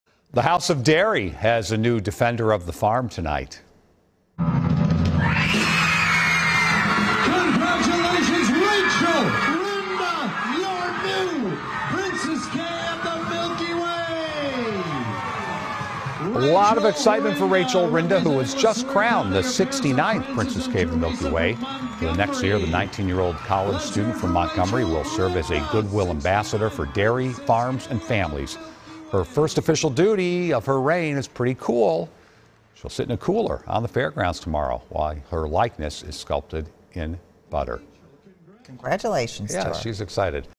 Rachel Rynda crowned 69th Princess Kay Of The Milky Way
On the eve of the 2022 Minnesota State Fair, a new princess reigns over Minnesota's dairy farms.